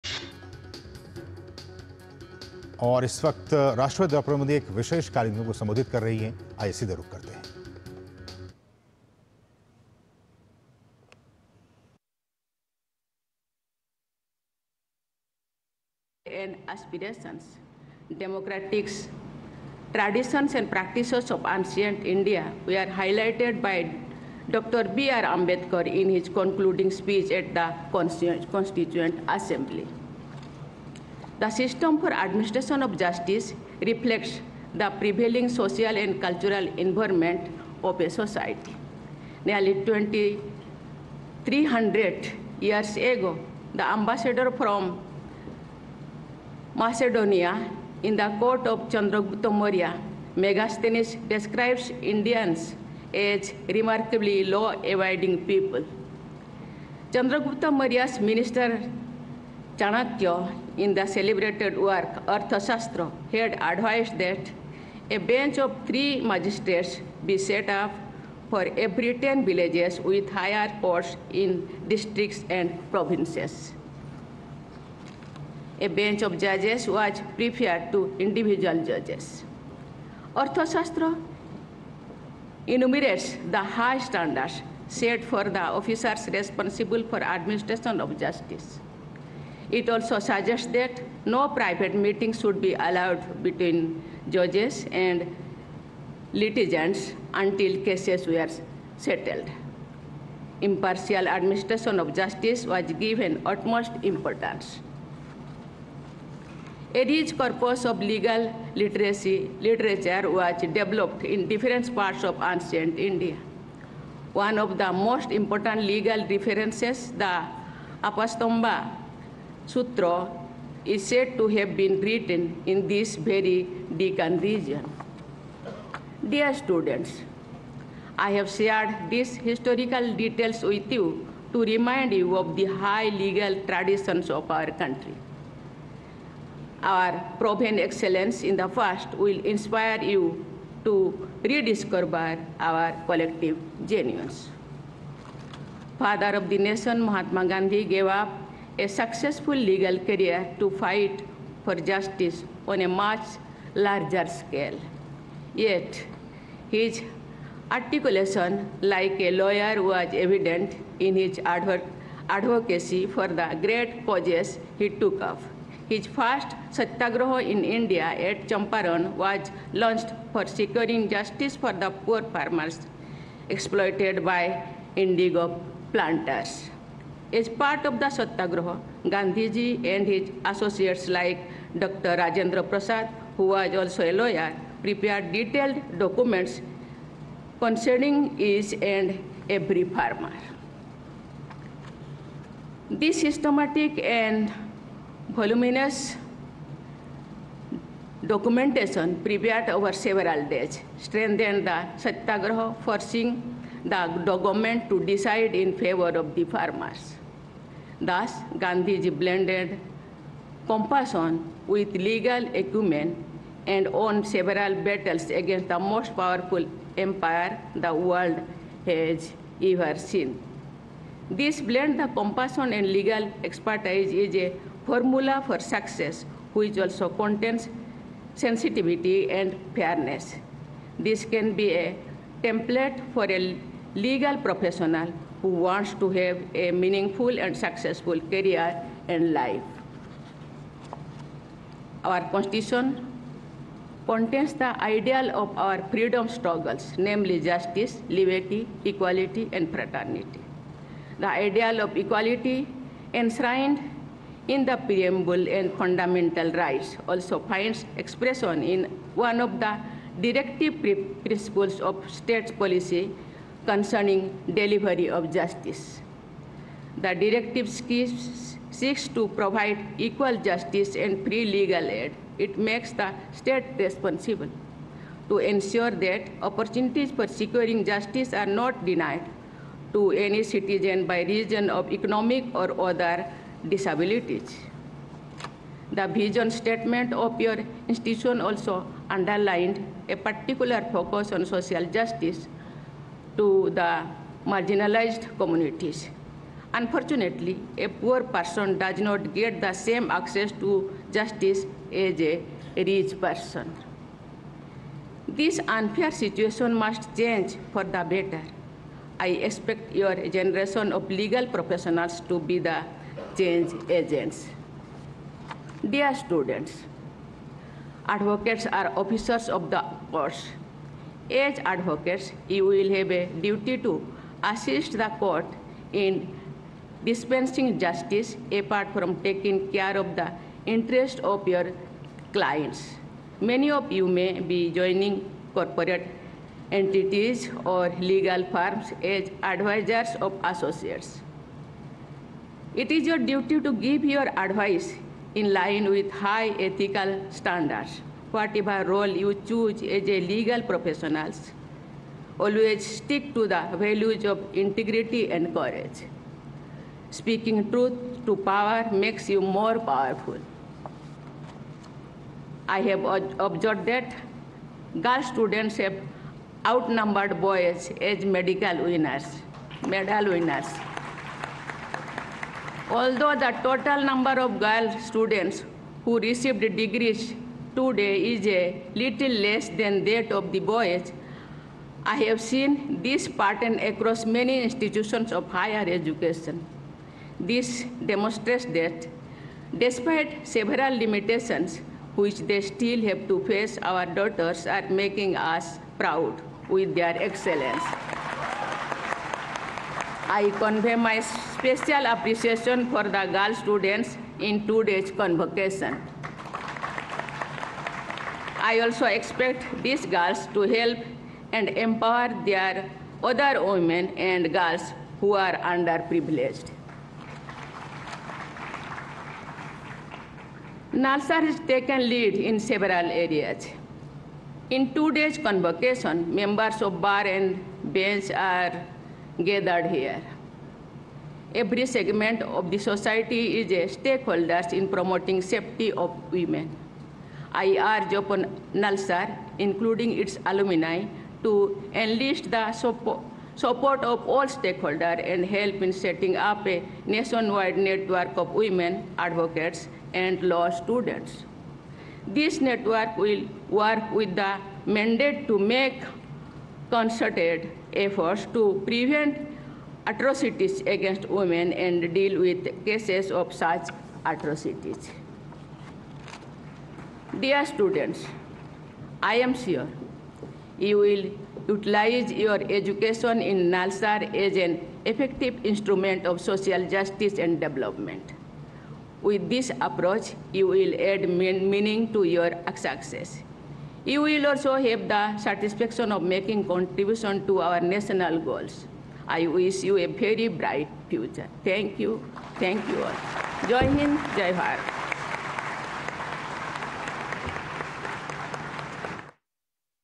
President Droupadi Murmu addressed the 21st convocation of NALSAR University in Hyderabad
🔍राष्ट्रपति द्रौपदी मुर्मू ने हैदराबाद में नेशनल एकेडमी ऑफ लीगल स्टडीज एंड रिसर्च (NALSAR) के 21वें वार्षिक दीक्षांत समारोह को संबोधित किया.